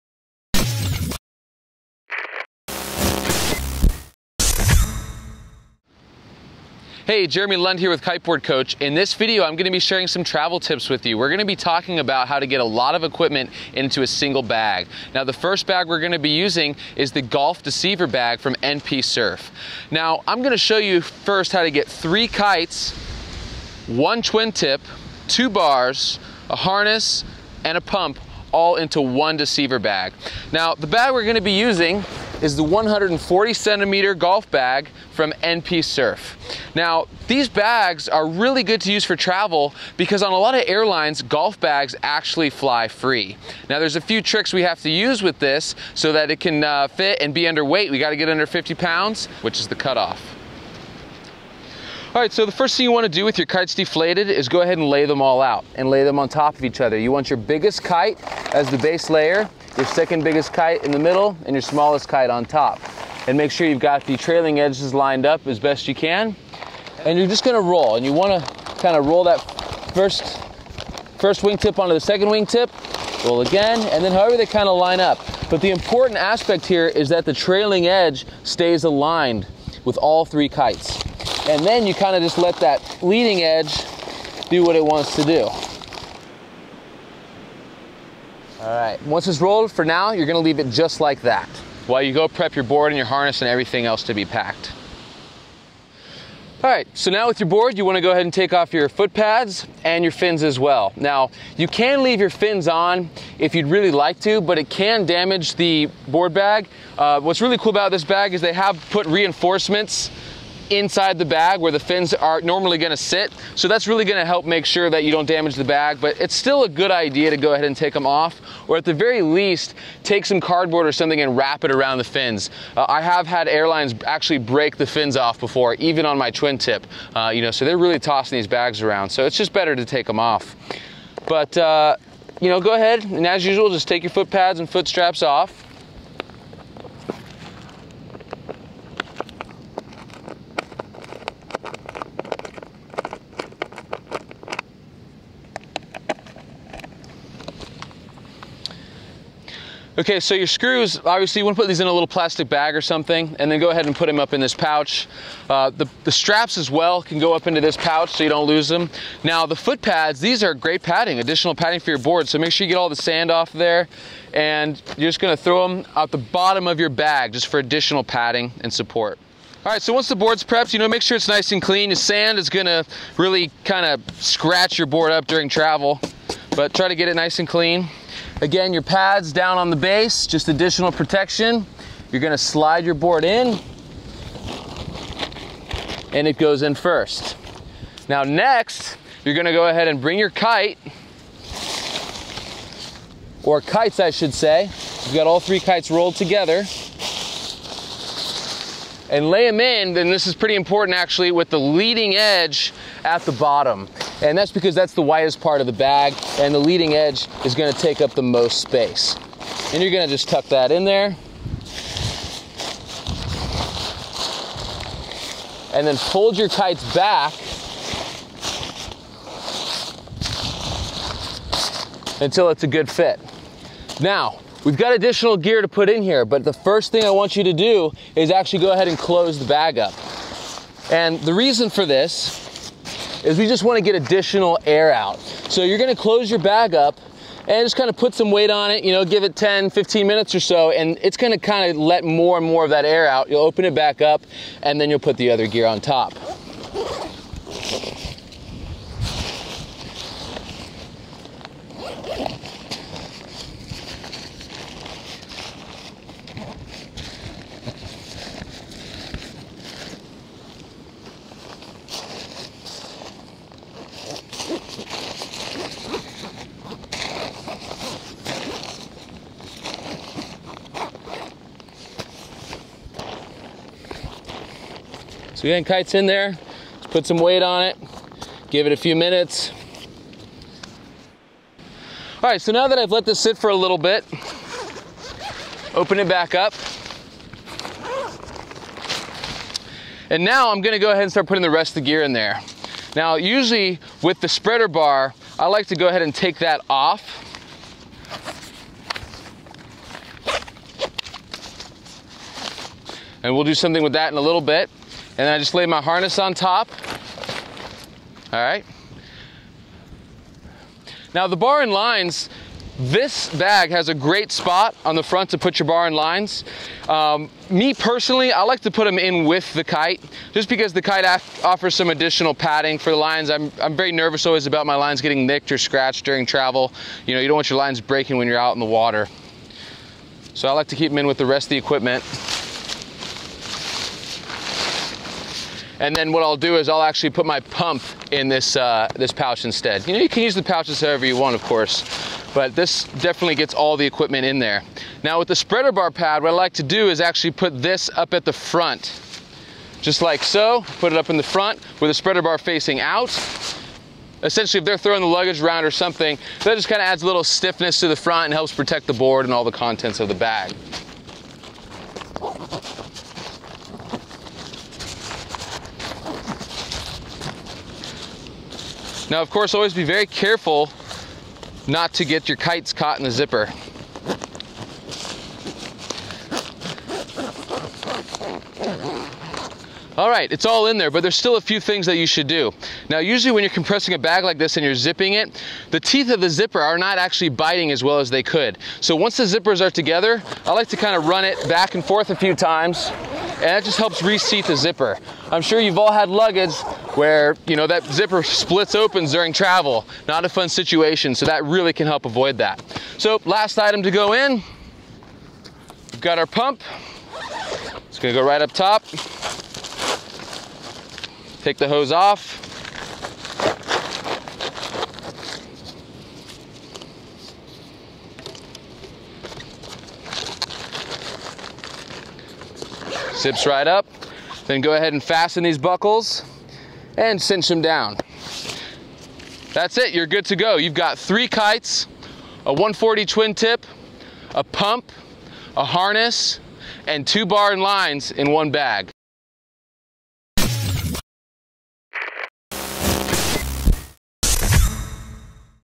How to pack your Kiteboarding equipment and save baggage fees
Jeremy Lund from Kiteboard Coach explains how to properly pack a deceiver (golf) bag for kiteboarding. Using a deceiver bag can usually allow you to travel with your kiteboarding equipment as normal baggage.